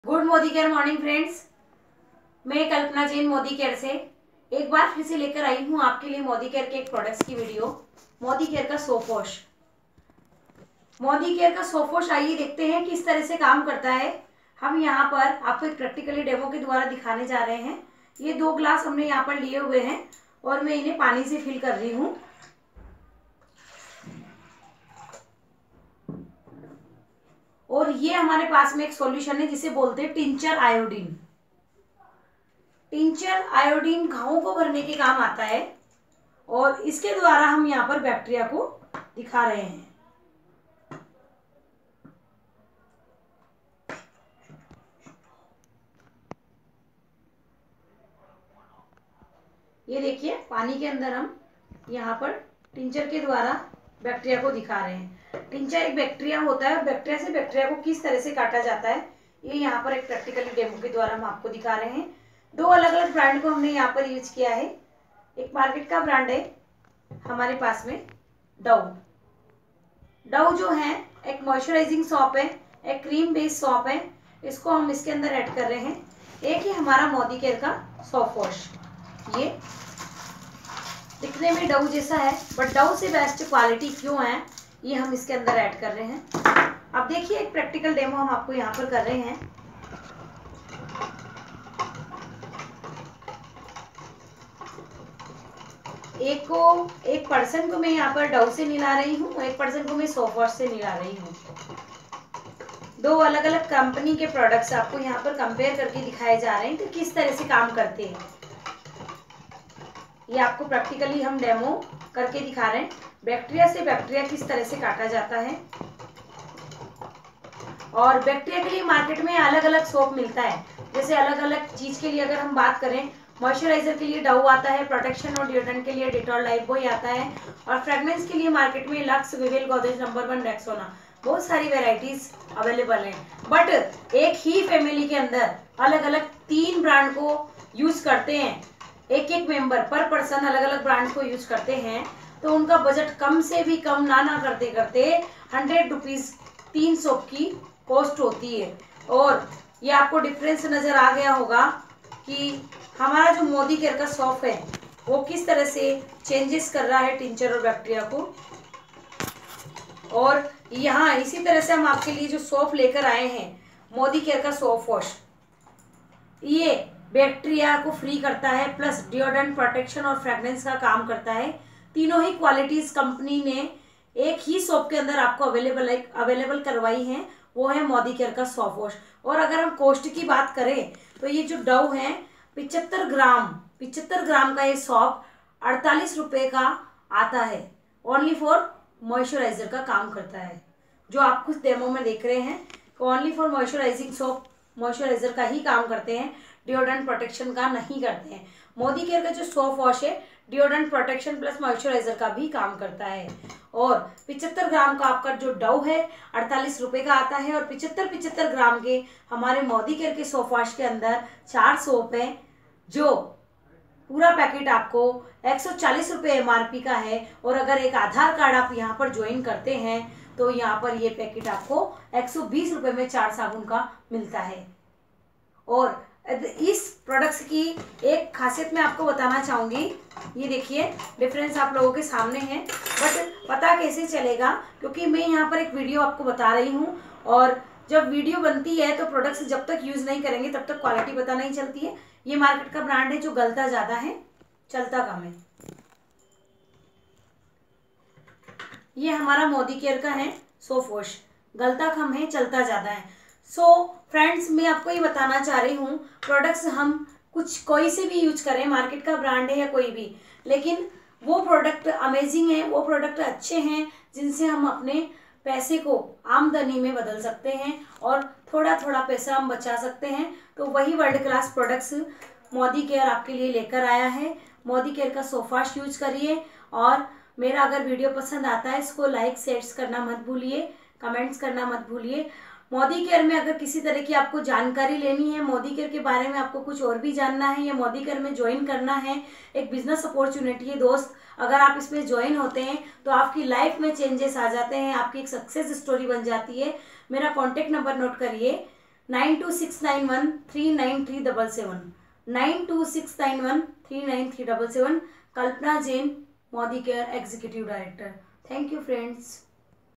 Modicare | Sofwash White Pearl Soap | Demo by Team Zealot's Kalpana Jain
SOFWASH WHITE PEARL SOAP (75G X 4)

Description
Gentle cleansing soap enriched with Glycerin and Germ Guards. Moisturizes and Deodorizes with rich creamy lather.